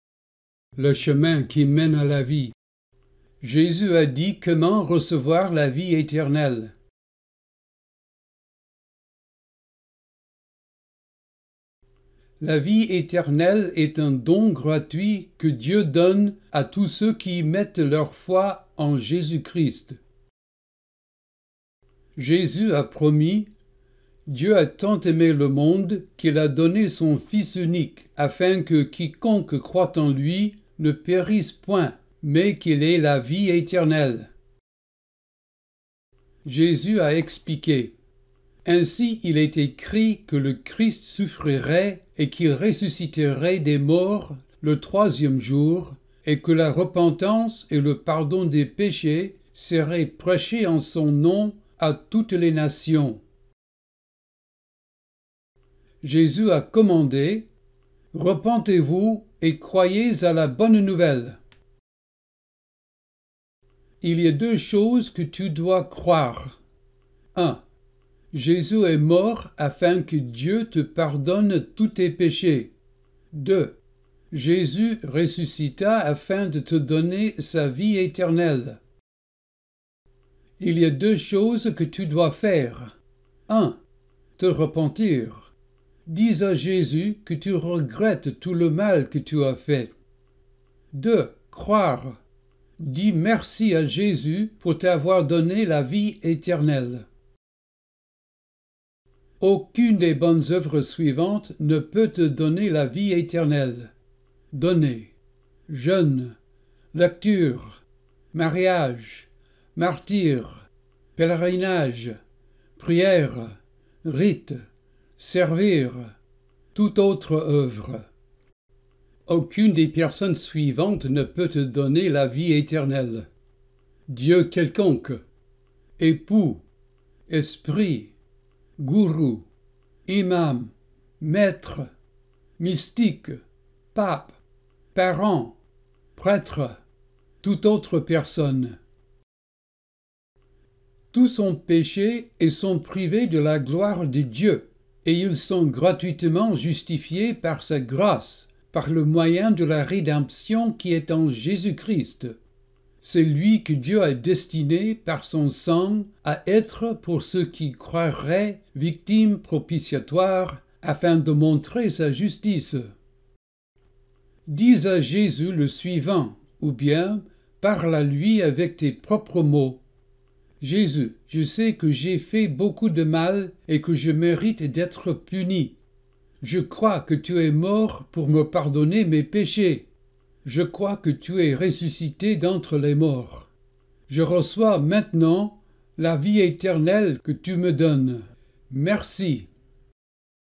« Le chemin qui mènent à la vie »
Jésus a dit comment recevoir la vie éternelle
Citations bibliques tirées de la version Louis Segond
Sans images et sans musique

La vie éternelle est un don gratuit que Dieu donne à tous ceux qui mettent leur foi en Jésus-Christ.

JÉSUS A PROMIS :

« Dieu a tant aimé le monde qu'il a donné son Fils unique, afin que quiconque croit en lui ne périsse point, mais qu'il ait la vie éternelle. » Sainte Bible : Jean 3 : 16

JÉSUS A EXPLIQUÉ :

« Ainsi il est écrit que le Christ souffrirait, et qu'il ressusciterait des morts le troisième jour, et que la repentance et le pardon des péchés seraient prêchés en son nom à toutes les nations. » Sainte Bible : Luc 24 : 44-45

JÉSUS A COMMANDÉ :

« Repentez-vous, et croyez à la bonne nouvelle. » Sainte Bible : Marc 1 : 15

Il y a deux choses que tu dois croire :

1. Jésus est mort afin que Dieu te pardonne tous tes péchés.

2. Jésus ressuscita afin de te donner de sa vie éternelle.

Il y a deux choses que tu dois faire :

1. Te repentir. Dis à Jésus que tu regrettes tout le mal que tu as fait.

2. Croire. Dis merci à Jésus pour t’avoir donné la vie éternelle.

Aucune des bonnes œuvres suivantes ne peut te donner la vie éternelle.

Donner, jeûnes, lectures, mariage, martyr, pèlerinages, prières, rites, servir, toute autre œuvre

Aucune des personnes suivantes ne peut te donner la vie éternelle.

Dieux quelconques, époux, esprits, gourous, imams, maîtres, mystiques, pape, parents, prêtres, tout autre personne

« Tous ont péché et sont privés de la gloire de Dieu; et ils sont gratuitement justifiés par sa grâce, par le moyen de la rédemption qui est en Jésus-Christ. C'est lui que Dieu a destiné, par son sang, à être, pour ceux qui croiraient victime propitiatoire, afin de montrer sa justice. » Sainte Bible : Romains 3:23-25

Dis à Jésus le suivant, ou bien parles à lui avec tes propres mots :

« Jésus, je sais que j’ai fait beaucoup de mal et que je mérite d’être puni. Je crois que tu es mort pour me pardonner mes péchés. Je crois que tu es ressuscité d’entre les morts. Je reçois, maintenant, la vie éternelle que tu me donnes. Merci. »